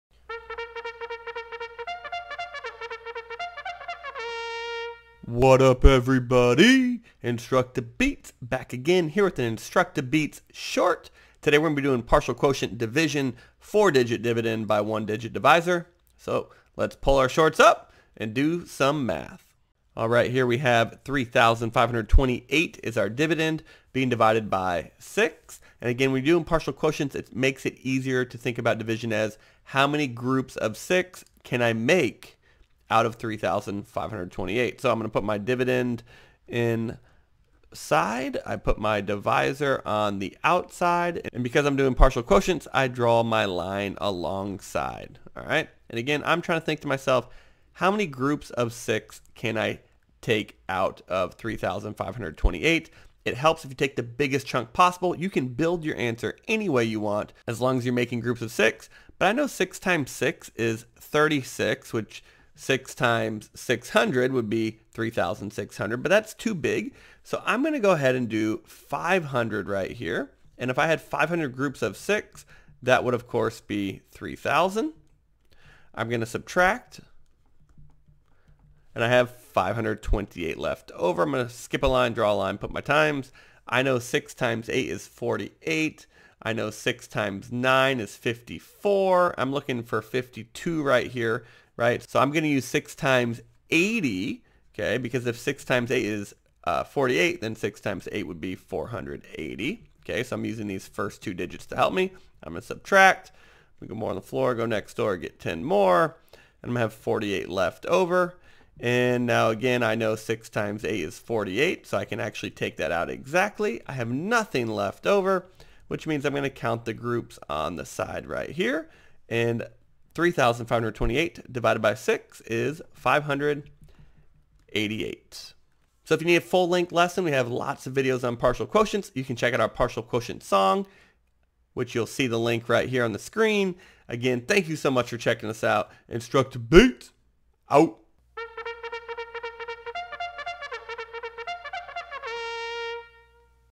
Partial Quotients 4 x 1 Problem - InstructaBeats Short
Check out this example problem in under 2 minutes! Check out this example problem in under 2 minutes!  We are using partial quotients to divide 4-digit by 1-digit numbers!  This quick short will demonstrate the partial quotients strategy!